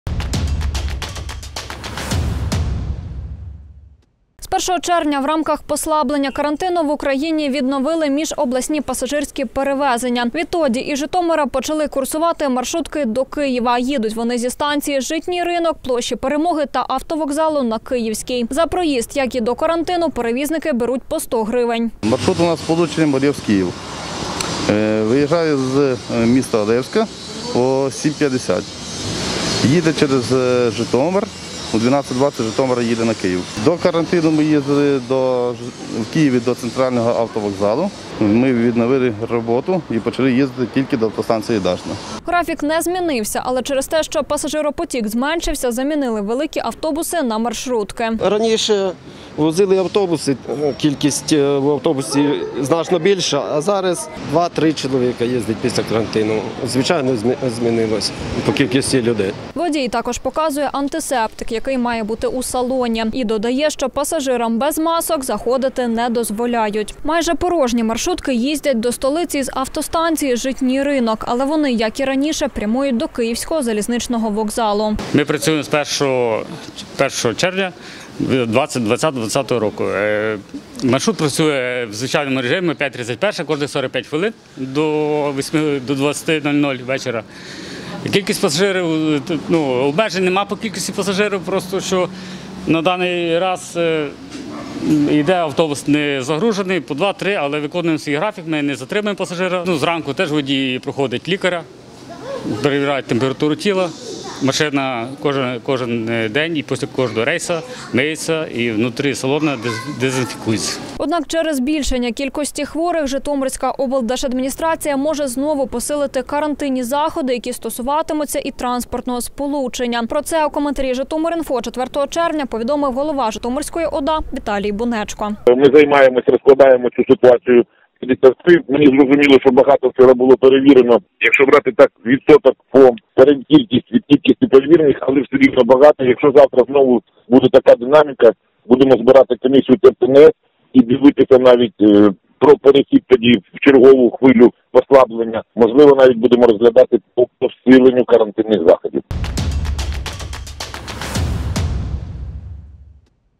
У Житомирі відновили автобусні маршрути до Києва - Житомир.info
З 1 червня в рамках послаблення карантину в Україні відновили міжобласні пасажирські перевезення. Відтоді із Житомира почали курсувати маршрутки до Києва, їдуть з автостанції «Житній ринок», площі Перемоги та автовокзалу на Київській. За проїзд, як і до карантину, перевізники беруть по 100 грн.